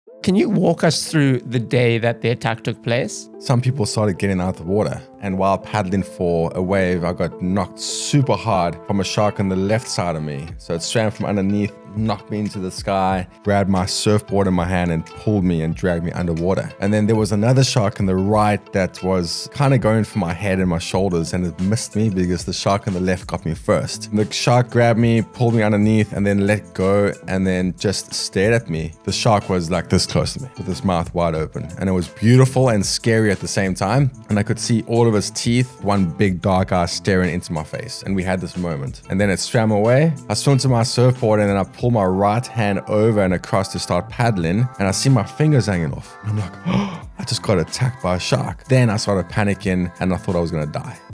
Surviving A Double Great White Shark Attack
In this clip from my podcast with Shannon Ainslie we talk about the time he was attacked by two Great White Sharks at the same time!